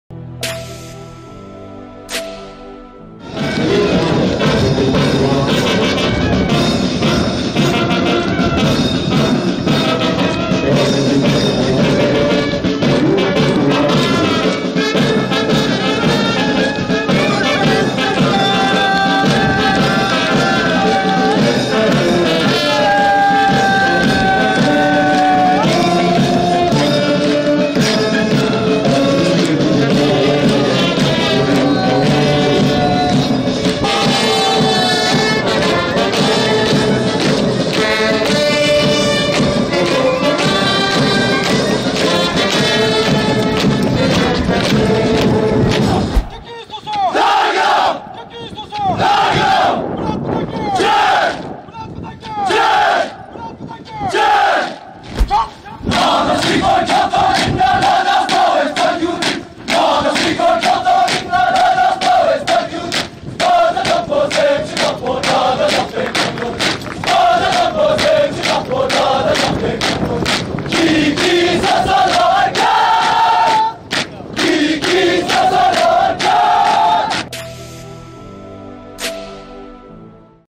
Ladakh Scouts Passing Parade 2021 | Indian Army | IMA
The Ladakh Scouts is an infantry regiment of the Indian Army, nicknamed the "Snow Warriors" or "Snow Leopards".The regiment specializes in mountain warfare, and its primary role is to guard India's borders in the high altitudes of the Union Territory of Ladakh.
Inspiration, determination, and rigorous training forge the human shields of Ladakh. The young nunus (Younger Brother} train at the highest regimental center in the world and are determined to serve their motherland. Their larger lungs are a boon for the lack of oxygen in the Ladakh region, where the recruits train at temperatures as low as -40 degrees!